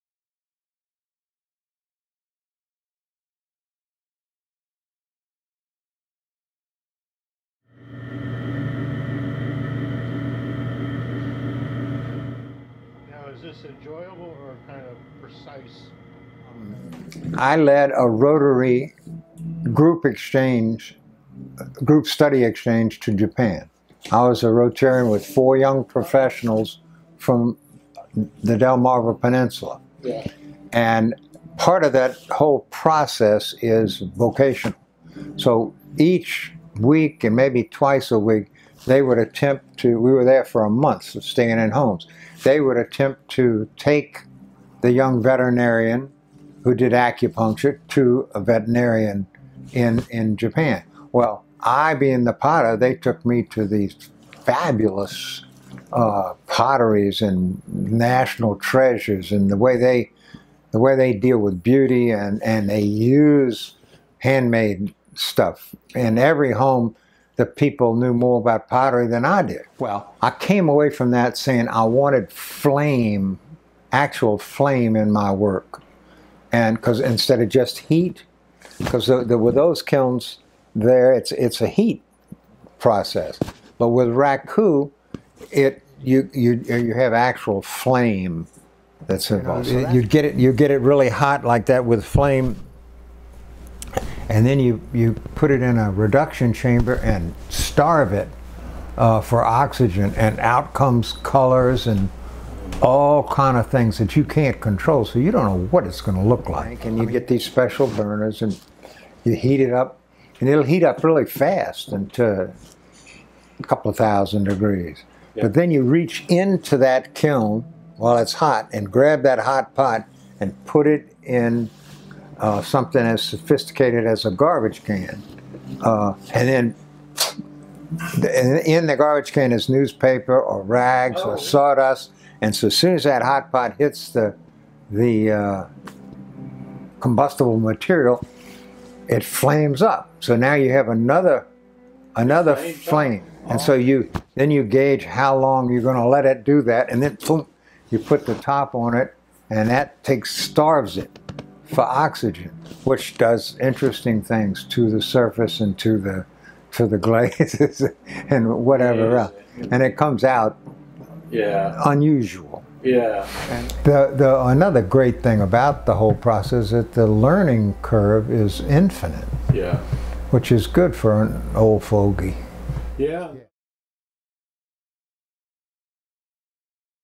Mid-Shore Arts: Potter Ken Sadler and the Pleasure of Raku
Ken Sadler's late life journey into pottery is a good story in itself. Discovering the art form from an accidental encounter with Paulus Berensohn's book, One's Way With Clay, when he was well into his late 60s, Sadler not only took up the potter's wheel but became totally absorbed with clay, including his passion for Raku pottery, the ancient Japanese method of using  direct flame to release its  special colors and exterior surfaces.

But it's not all "high art" with Ken's work. Perhaps influenced by his clown character, Dr. Goodwrench, who has appeared at the Easton hospital for years, he has a complete line of clay snakes (and other garden art) that are realistic enough to scare one's favorite Eastern Shore gardener, or more recently, his creation of "Watchers," figures designed to be his modern version the St. Christopher medal; very capable of  special protection for its owners.

When Ken and his wife, Sarah, recently sold their Oxford house and moved into Londonderry on the Tred Avon, Ken transferred his studio to the Davis Art Center in Easton to continue both his serious and not so serious pottery. That's where the Spy found him last month and we had a brief conversation with him about the special pleasures that come with Raku.